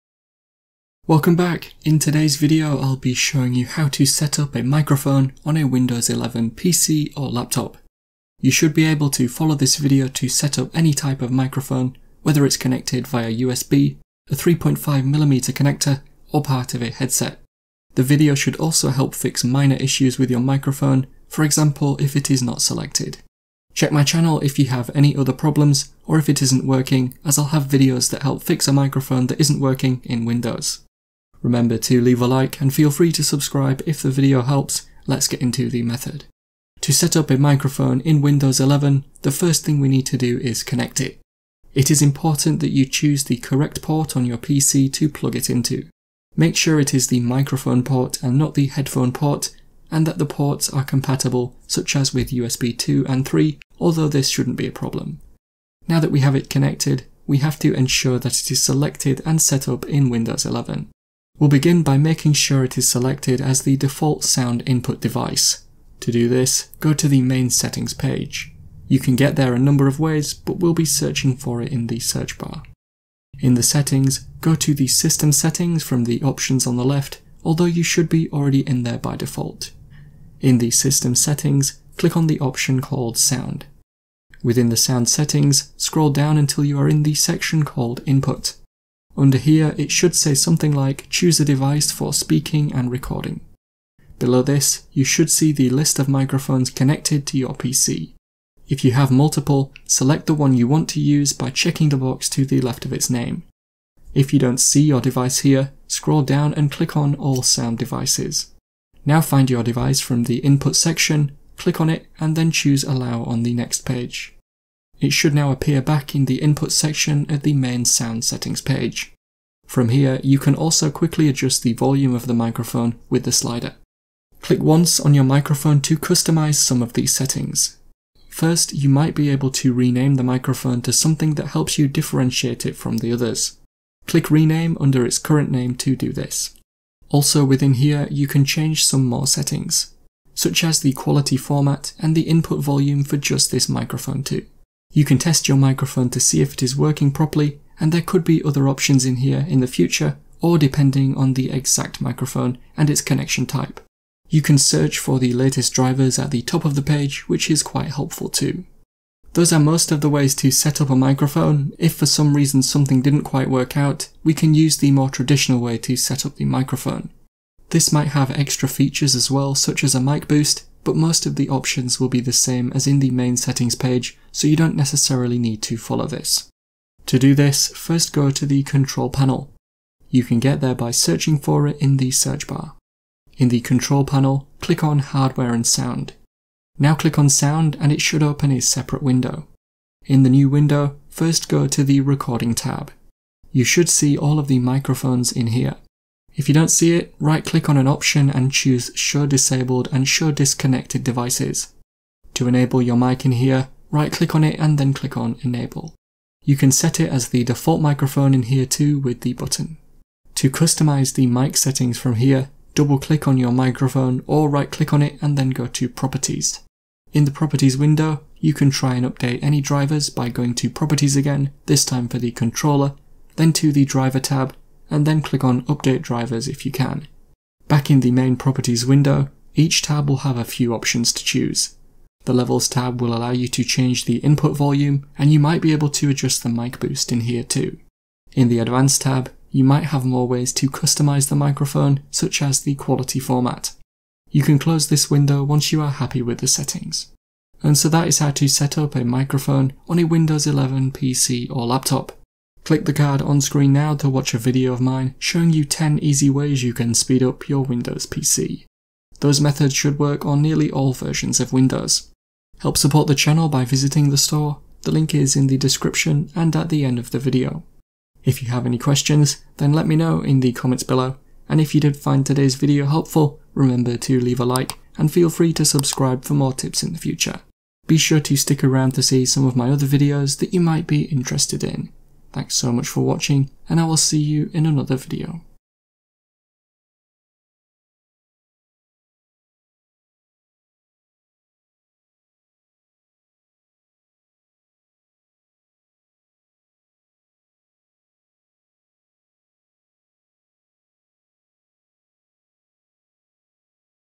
How to Set Up a Microphone on Windows 11 PC & Laptop
Want to know how to set up a microphone on a Windows 10 PC or laptop? This video will show you how to setup a mic or microphone in Windows 11. You might want to know how to set up a microphone/headset on Windows 11 if your microphone is not working in Windows 11. We'll be focusing on how to set up a microphone and customize the sound, such as the mic boost. Hopefully today's video helps show you how to connect a microphone on a Windows 11 PC or laptop!